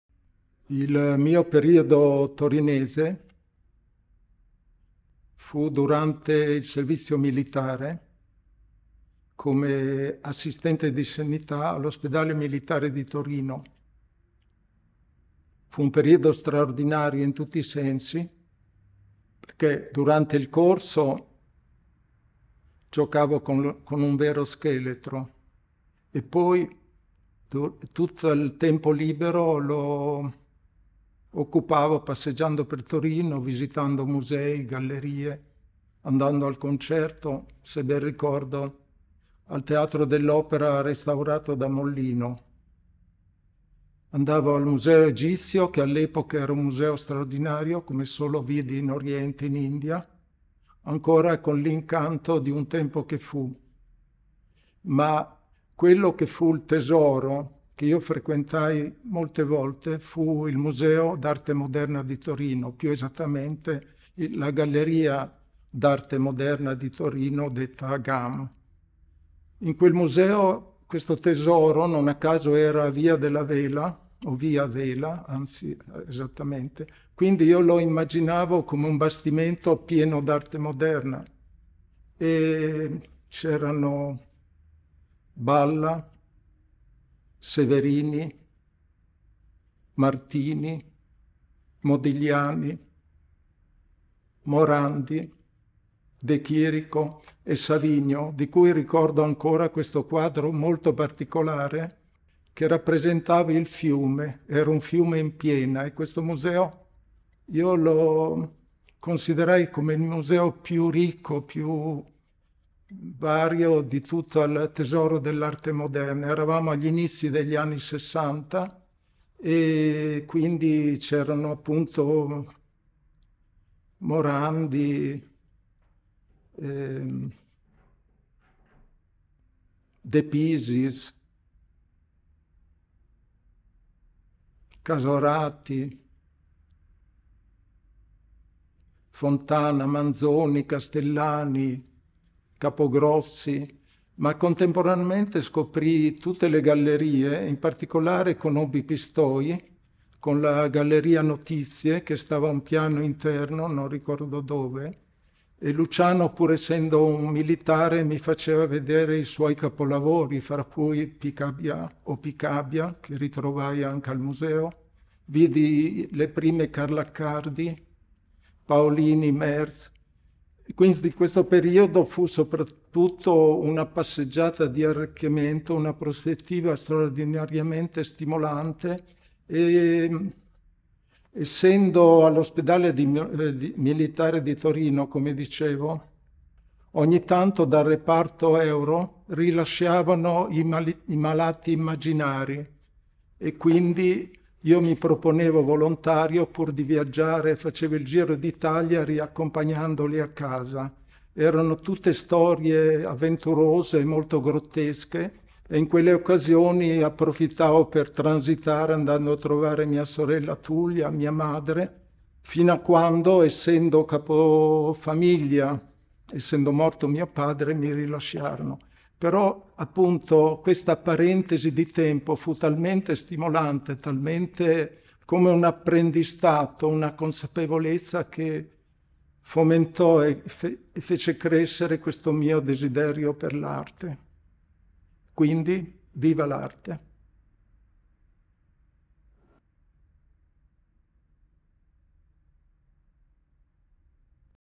Luigi Ontani per GAM Torino
"L’arte con chi ne fa parte" è la nuova iniziativa di Abbonamento Musei che ti porta alla scoperta delle collezioni dei musei piemontesi e valdostani, con occhi nuovi: saranno infatti i fondatori, i primi collezionisti, gli imprenditori, i nobili e i sovrani che li hanno abitati a condurti lungo suggestivi percorsi di visita. Il cicerone della GAM è l'artista Luigi Ontani.